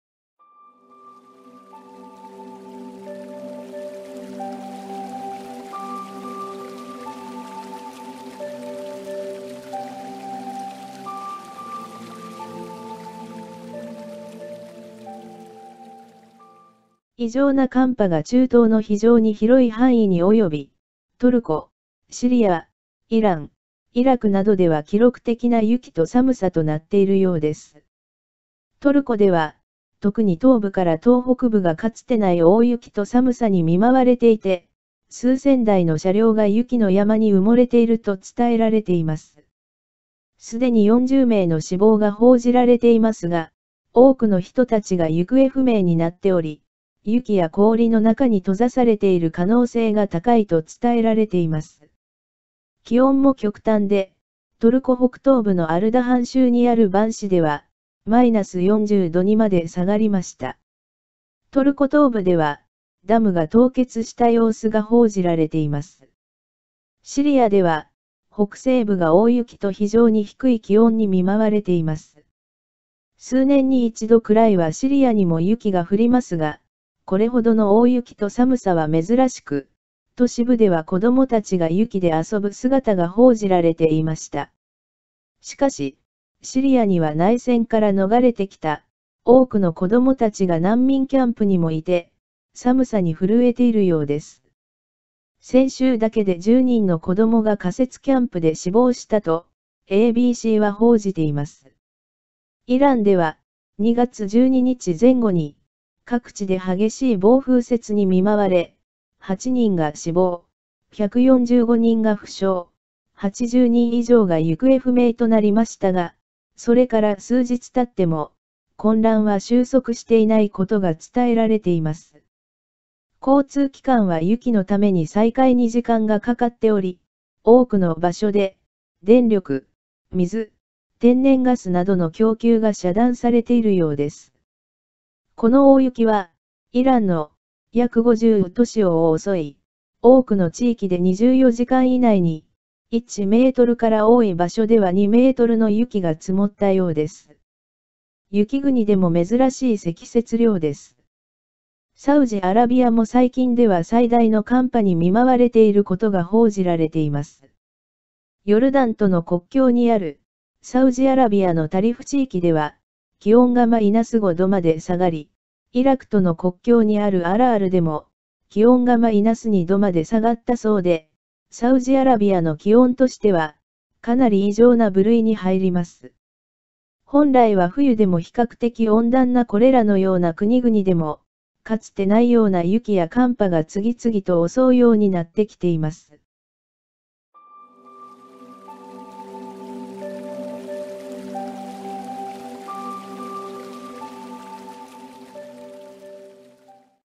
中東では異常低温が続いており壊滅的な大雪と寒波に襲われている
異常な寒波が中東の非常に広い範囲に及び、トルコ、シリア、イラン、イラクなどでは記録的な雪と寒さとなっているようです。トルコでは、特に東部から東北部がかつてない大雪と寒さに見舞われていて、数千台の車両が雪の山に埋もれていると伝えられています。すでに40名の死亡が報じられていますが、多くの人たちが行方不明になっており、雪や氷の中に閉ざされている可能性が高いと伝えられています。気温も極端で、トルコ北東部のアルダハン州にあるヴァン市では、-40℃にまで下がりました・・・

【日本ではあまり報じられない世界のニュースから】